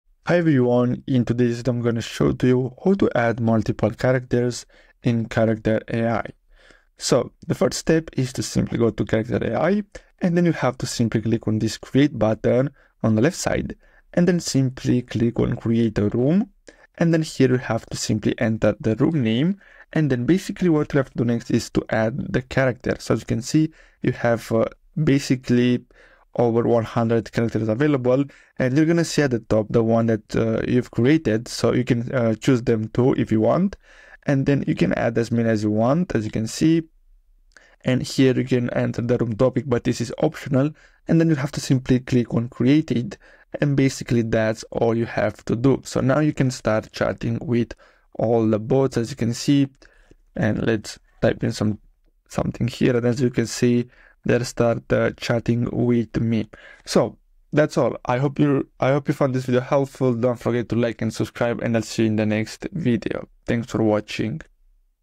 How To Add Multiple Characters In Character AI (Step by Step)
Learn how to make money for free Adding multiple characters to a character AI system can greatly enhance its capabilities and make it more realistic and engaging for users. The first step in this process is to define the characteristics and behaviors of each character, including their personality traits and interactions with other characters.

Next, the AI must be trained to recognize and respond to each character individually, through the use of natural language processing and machine learning techniques. Finally, the AI must be programmed to dynamically balance and prioritize the actions and dialogue of multiple characters, creating a seamless and immersive experience for users. With careful planning and technical expertise, the addition of multiple characters to a character AI system can greatly enhance its performance and user experience.